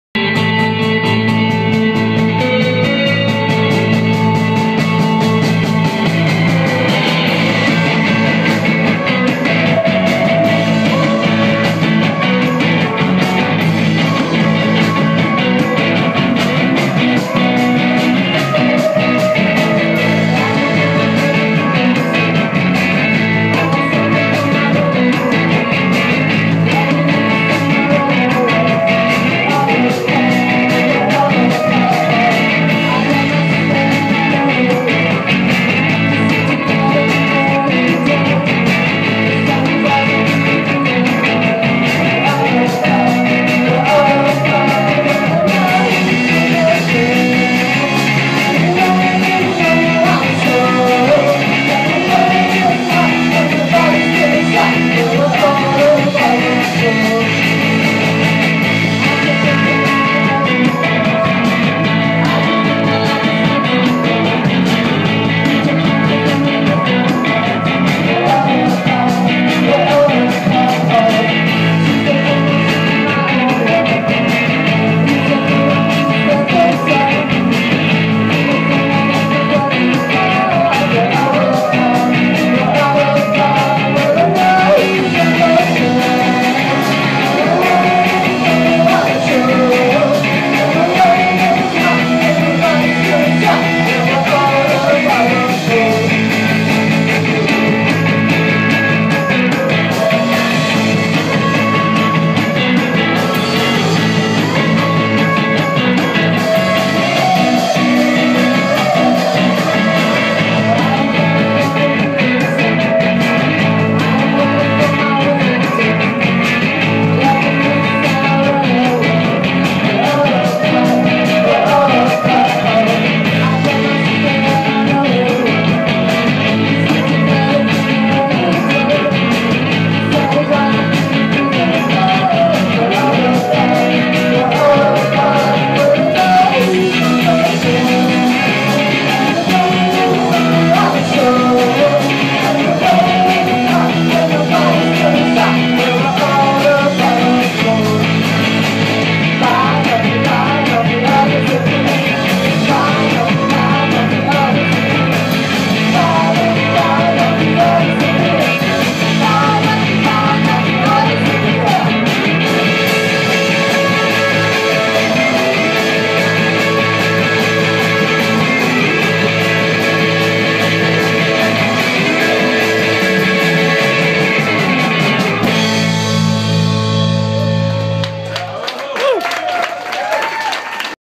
JUNIPER RISING - new song @ Union Pool, Brooklyn, May 4, 2013
Juniper Rising opened this show with Organs and The Men.